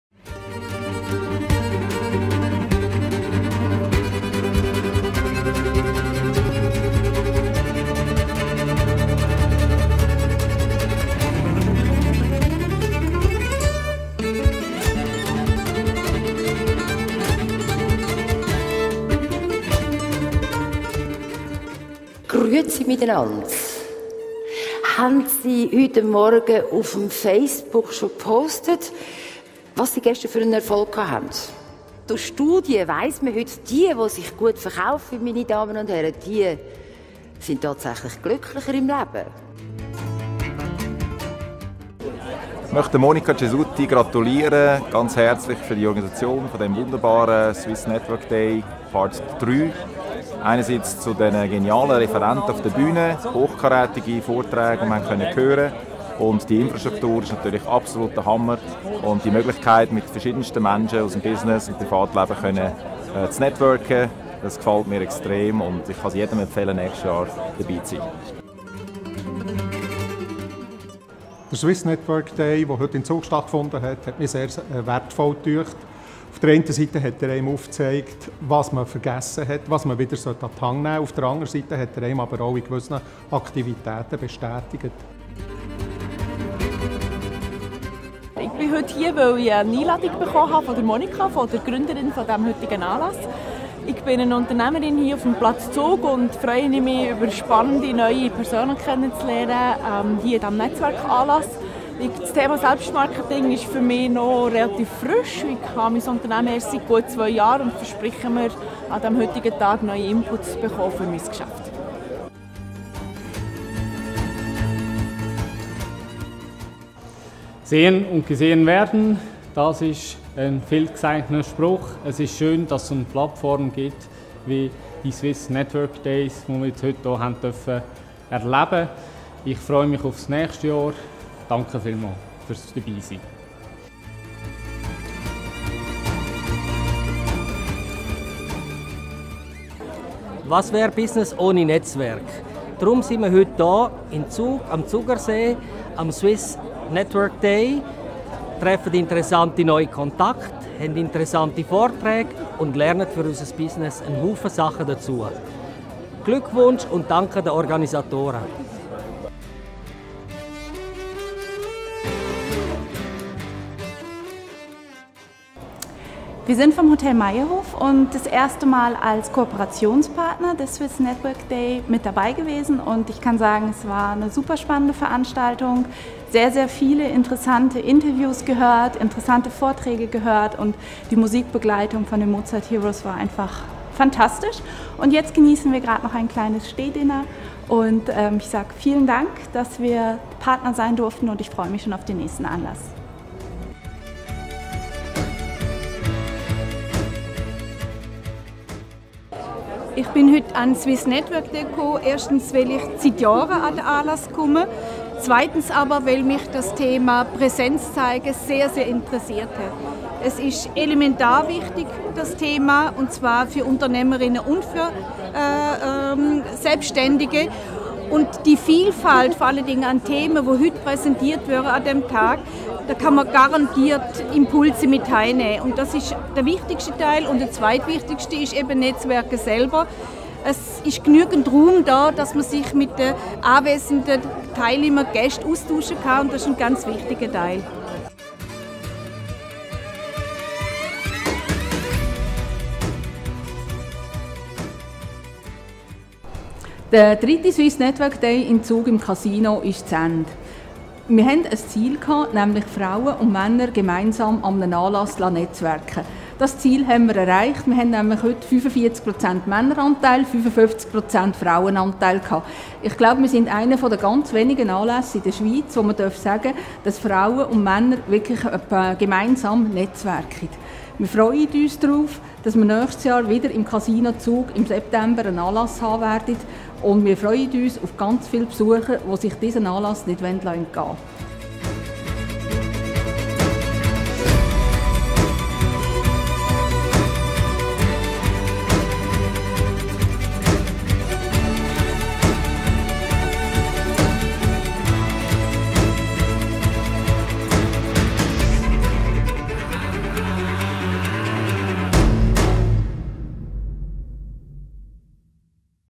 Swiss Network Day 25.05.16
Endlich ist der Film vom letzten Swiss Network Day vom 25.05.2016 online. Vielen Dank an Petra Odermatt von Odermatt Films +. Der nächste Swiss Network Day findet am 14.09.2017 statt, weitere Informationen